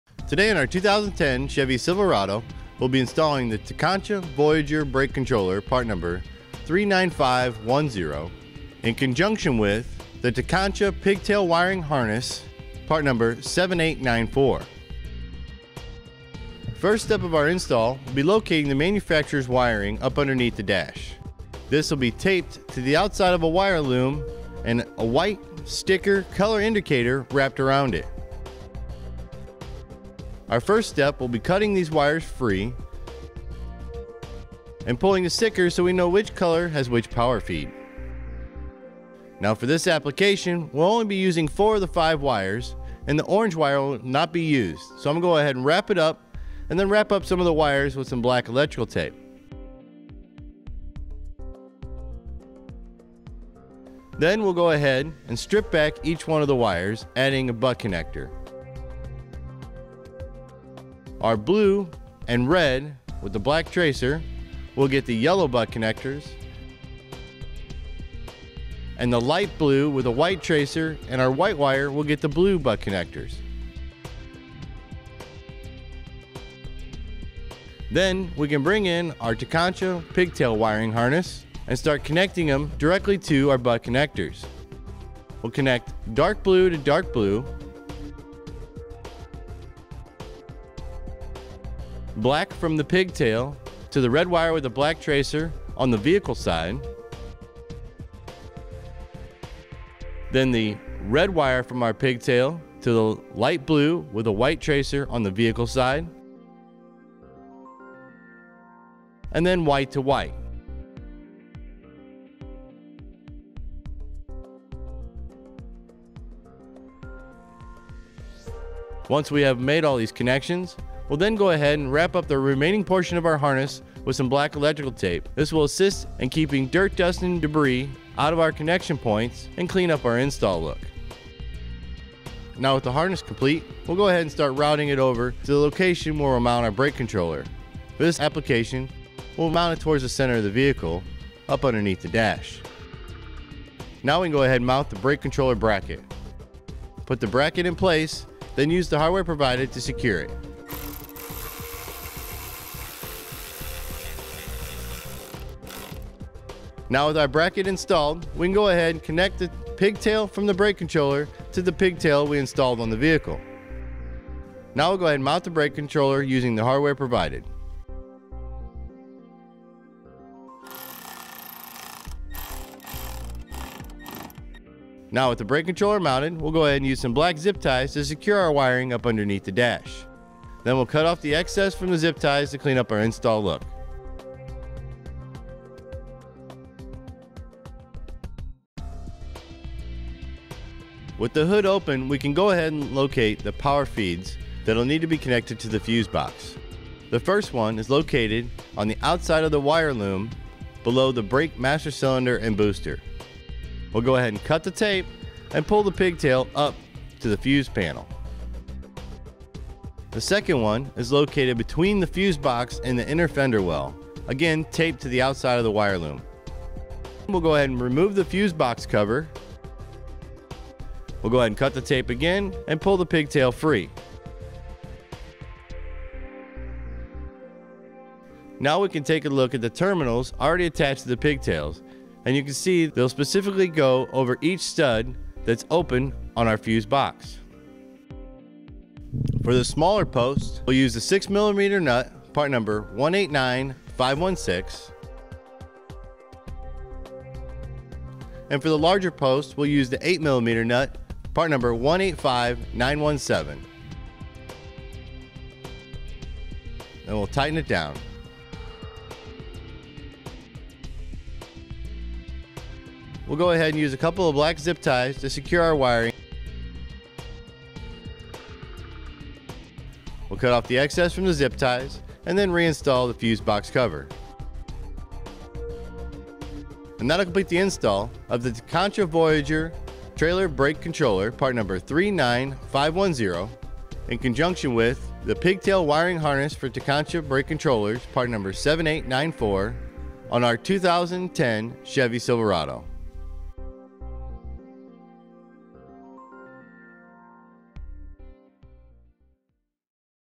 How to Install: Tekonsha Voyager Trailer Brake Controller on your 2010 Chevrolet Silverado
Today on our 2010 Chevrolet Silverado well be installing the Tekonsha Voyager Brake Controller, part number 39510 in conjunction with the Tekonsha Pigtail Wiring Harness, part number 7894.  The first step of our install will be locating the manufacturers wiring up underneath the dash.  This will be taped to the outside of a wire loom and a white sticker color indicator wrapped around it.  Our first step will be cutting these wires free and pulling the sticker so we know which color has which power feed.  Now for this application well only be using four of the five wires and the orange wire will not be used so Im going to go ahead and wrap it up then wrap up some of the wires with some black electrical tape.  Then well go ahead and strip back each one of the wires adding a butt connector.

Our blue and red with the black tracer will get the yellow butt connectors, and the light blue with the white tracer and our white wire will get the blue butt connectors.  Then we can bring in our Tekonsha Pigtail Wiring Harness and start connecting them directly to our butt connectors.  Well connect dark blue to dark blue, black from the pigtail to the red wire with the black tracer on the vehicle side, then the red wire from our pigtail to the light blue with the white tracer on the vehicle side, and then white to white.  Once we have made all these connections, well then go ahead and wrap up the remaining portion of our harness with some black electrical tape.  This will assist in keeping dirt, dust, and debris out of our connection points and clean up our install look.

Now with the harness complete well go ahead and start routing it over to the location where well mount our brake controller.  For this application, well mount it towards the center of the vehicle up underneath the dash.  Now we can go ahead and mount the brake controller bracket.  Put the bracket in place, then use the hardware provided to secure it.  Now with our bracket installed, we can go ahead and connect the pigtail from the brake controller to the pigtail we installed on the vehicle.

Now well go ahead and mount the brake controller using the hardware provided.  Now with the brake controller mounted well go ahead and use some black zip ties to secure our wiring up underneath the dash.  Then well cut off the excess from the zip ties to clean up our install look.  With the hood open we can go ahead and locate the power feeds thatll need to be connected to the fuse box.  The first one is located on the outside of the wire loom below the brake master cylinder and booster.

Well go ahead and cut the tape and pull the pigtail up to the fuse panel.  The second one is located between the fuse box and the inner fender well again taped to the outside of the wire loom.  Well go ahead and remove the fuse box cover.  Well go ahead and cut the tape again and pull the pigtail free.  Now we can take a look at the terminals already attached to the pigtails, and you can see theyll specifically go over each stud thats open on our fuse box.  For the smaller posts, well use a 6 mm nut, part number 189516, and for the larger posts well use the 8 mm nut, part number 18591,7 and well tighten it down.  Well go ahead and use a couple of black zip ties to secure our wiring.  Well cut off the excess from the zip ties and then reinstall the fuse box cover.  And that will complete the install of the Tekonsha Voyager Trailer Brake Controller, part number 39510 in conjunction with the Pigtail Wiring Harness for Tekonsha Brake Controllers, part number 7894 on our 2010 Chevy Silverado. .